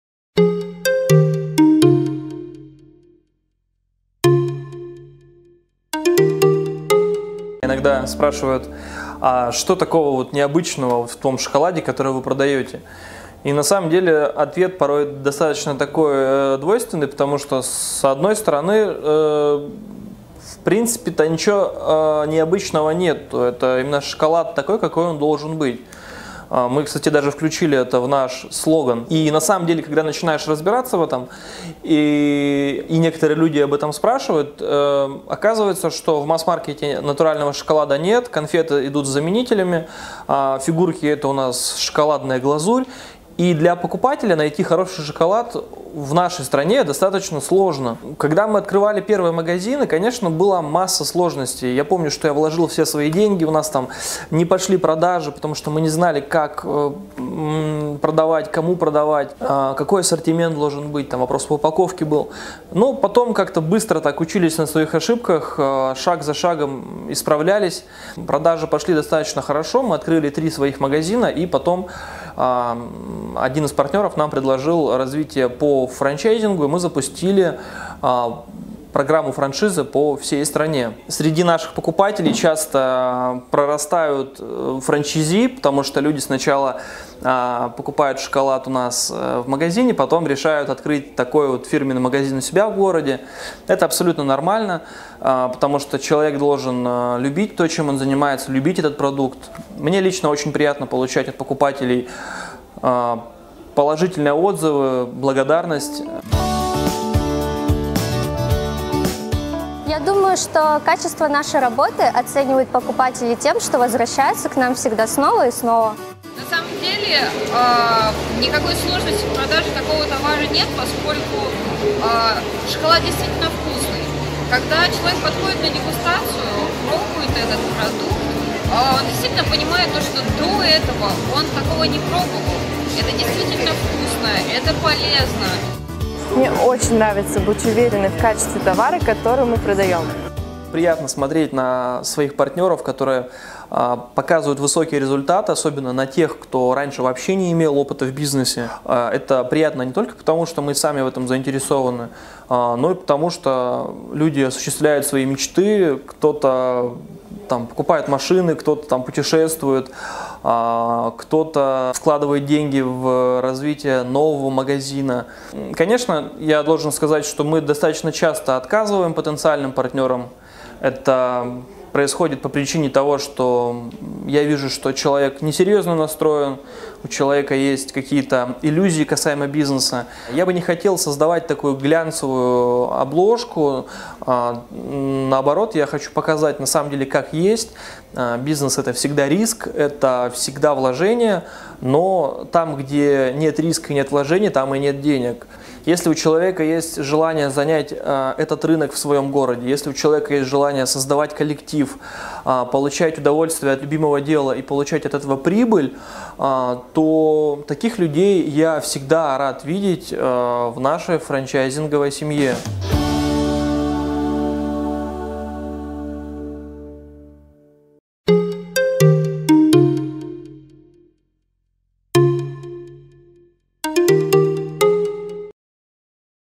Франшиза Delicert de chocolat
Delicert de chocolat - уникальная для российского рынка франчайзинговая сеть магазинов натурального шоколада работающих в формате островков в торговых центрах. Открыто 12 магазинов в 9 городах (на август 2017). В среднем 3 магазина всегда находится на стадии открытия.